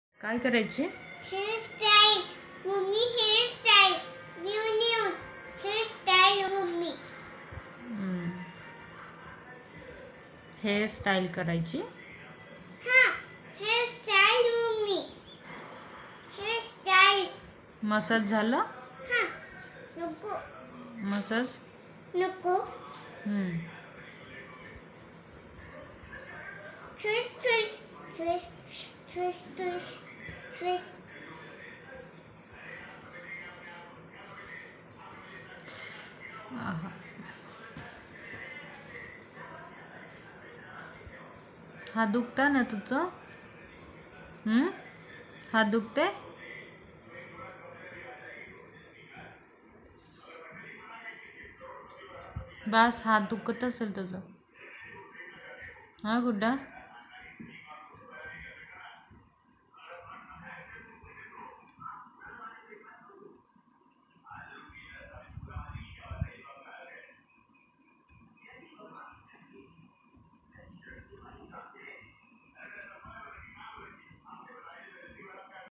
🥰 मां को कितना ख्याल रखती हैं, मेरी बेटी मेरे बालोंका मसाज and हेयर स्टाइल कर रही है 😰
🥰 मां को कितना ख्याल रखती हैं, मेरी बेटी मेरे बालोंका मसाज and हेयर स्टाइल कर रही है 😰 कितना प्यार करती love you जान एम#viral